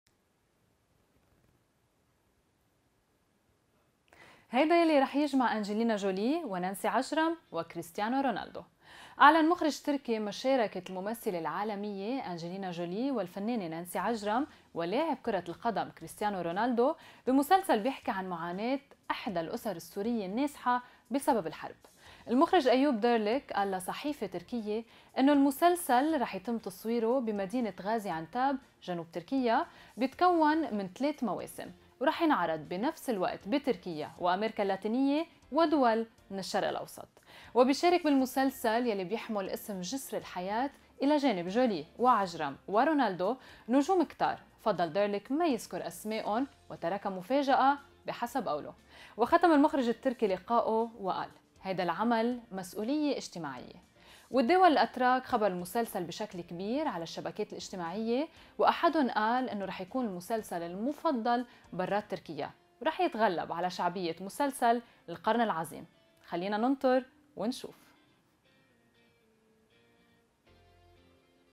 ما الذي سيجمع أنجلينا جولي ونانسي عجرم وكريستيانو رونالدو ؟
أعلن مخرج تركي مشاركة الممثلة العالمية أنجلينا جولي والفنانة نانسي عجرم ولاعب كرة القدم كريستيانو رونالدو في مسلسل يروي معاناة إحدى الأسر السورية النازحة بسبب الحرب.